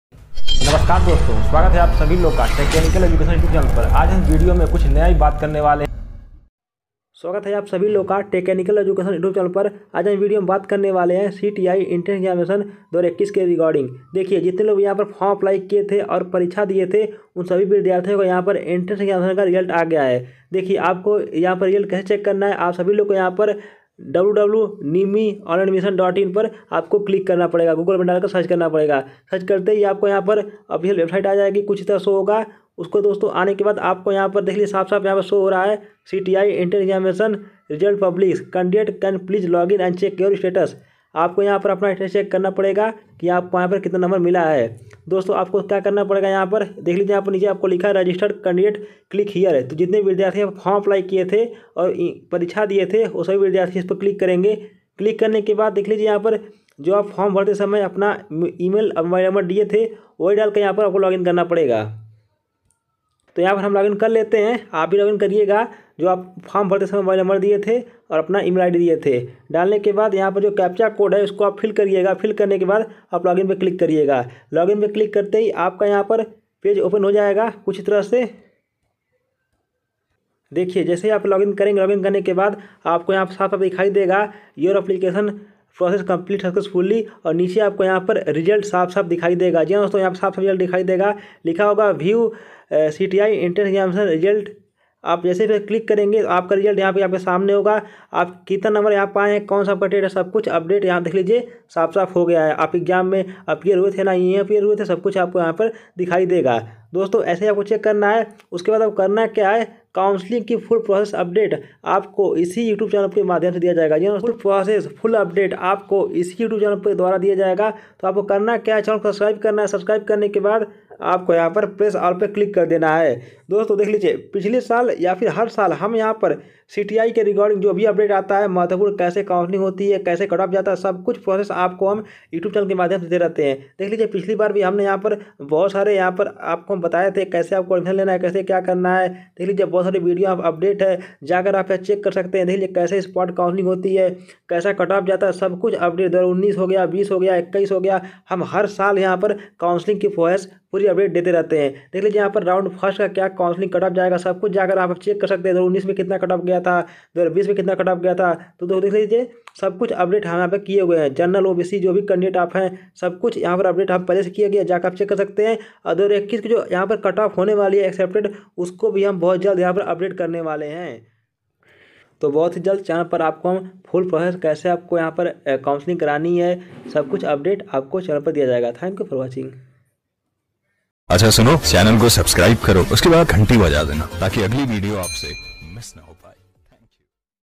CITS Entrance Exam 2021 Result Declared || CTI प्रवेश परीक्षा का रिजल्ट आ गया
note- जल्द ही काउंसलिंग की प्रोसेस शुरू हो जाएगी

Second Round counciling Expected CUT-OFF जानने के लिए channel के साथ जुड़े रहे

CTI/CITS entrance exam 2021,Check Result,cti/cits का रिजल्ट कैसे देखें

 Notice-:any query contact whatapp Only